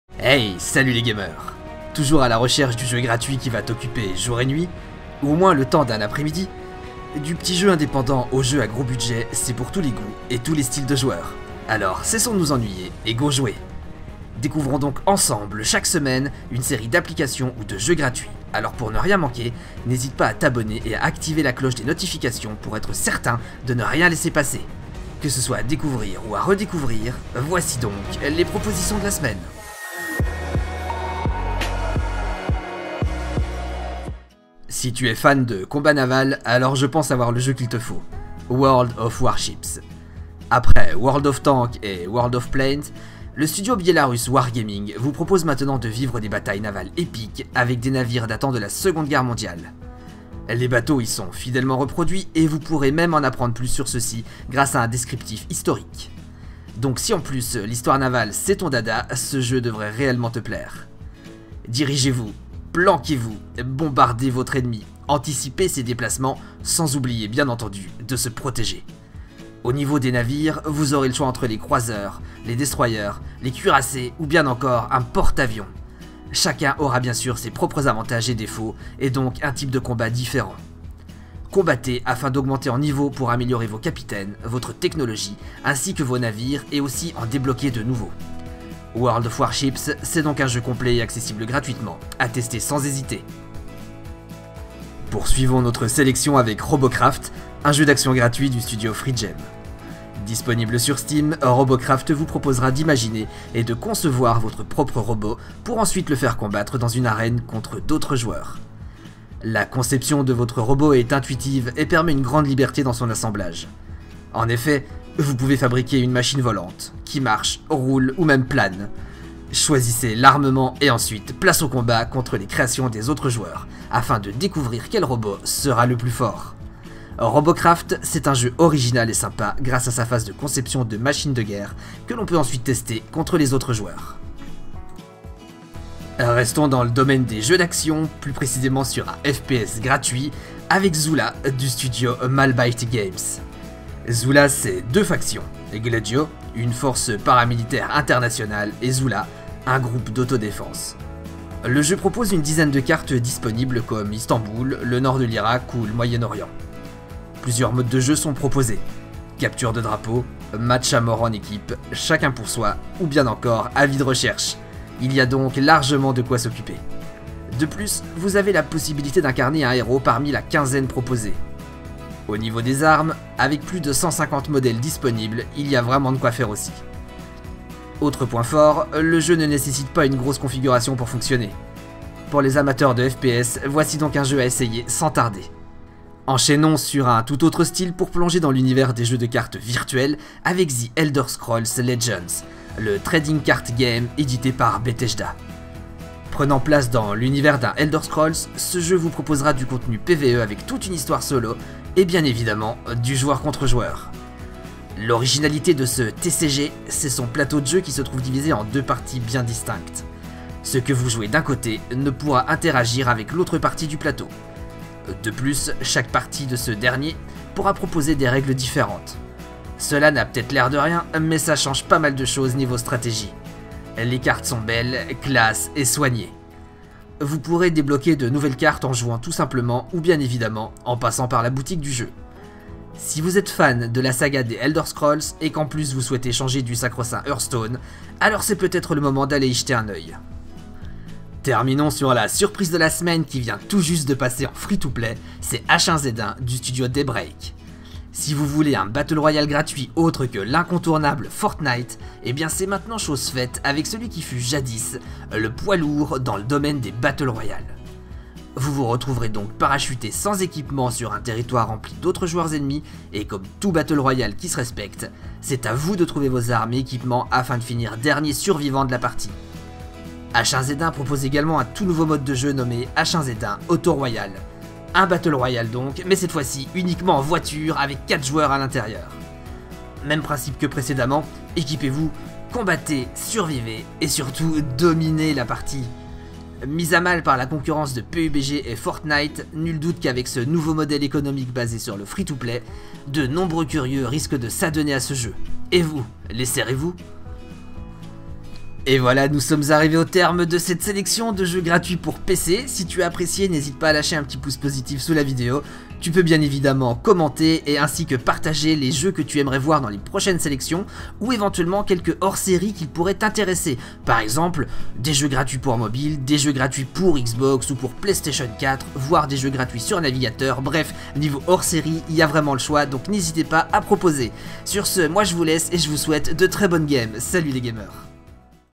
TOP JEUX GRATUITS 2018 #5 - Une sélection de jeux F2P pour PC !
Tu recherches un jeu gratuit ? Voici LA sélection de jeux Free To Play pour pc, notre nouveau Rendez-Vous hebdomadaire !